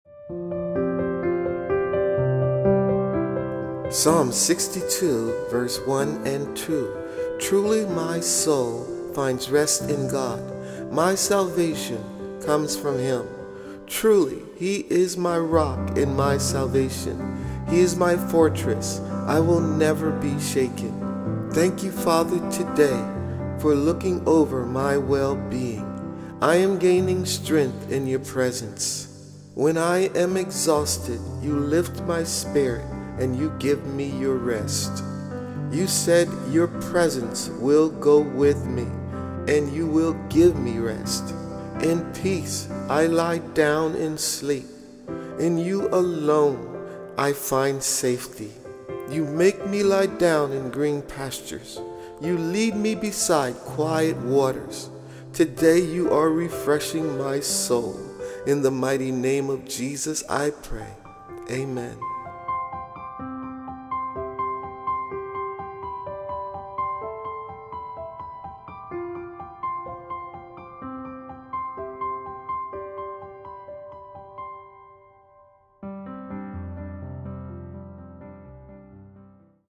🙏🏻 Truly My Soul Finds Rest In God (Psalm 62:1,2) Wednesday September 20, 2023
When we enter God's rest, we rest from our work, just like God rested from His. We rest from the exhausting, futile, unpleasant work of trying to earn our way to heaven. We rest from the desire to prove our righteousness to God over and over again. 🔥